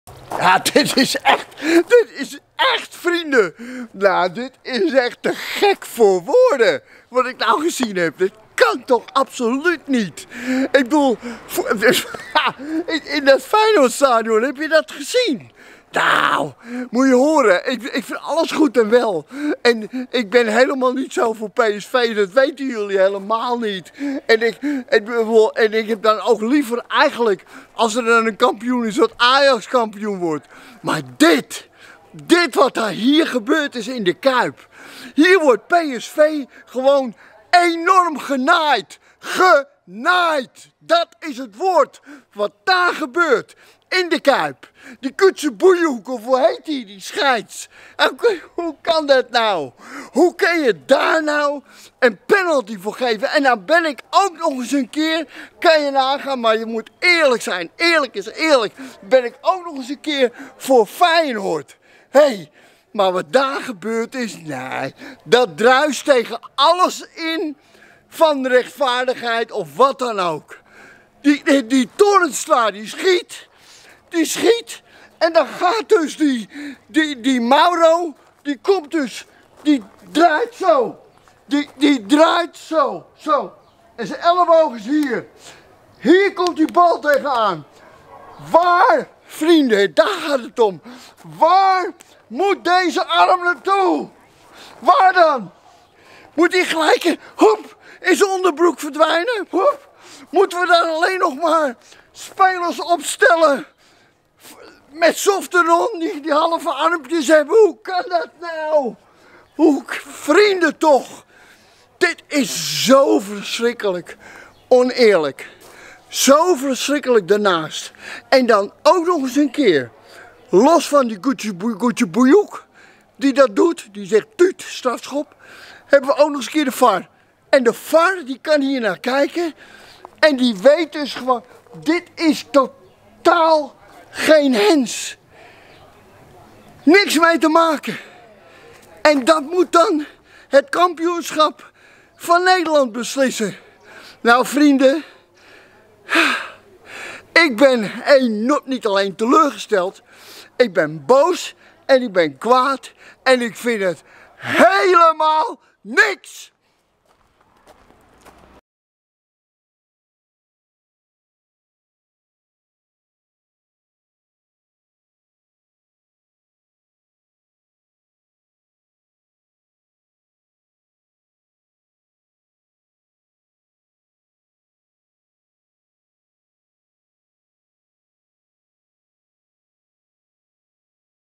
Wat nou strafschop bij Feyenoord PSV!! Wat een schande!
Wat een grof schandaal in De Kuip! PSV is gewoon bestolen. Hoezo een stafschop! En zo wordt dan het kampioenschap van Nederland beslist. Dankjewel scheidsrechter Gözübüyük en de VAR!